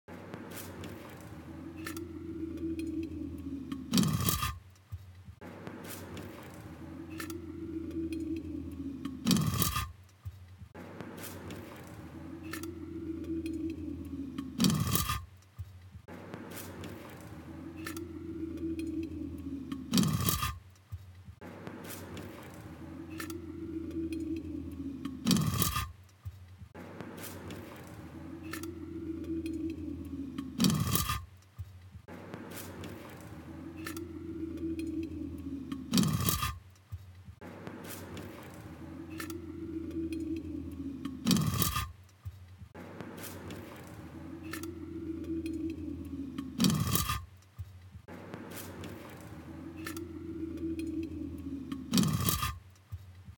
VR Fishbowl Covering 360 Camera Loop
A loop of myself putting a fishbowl on top of a 360° camera — a v disorienting experience

Made with the Ricoh Theta V.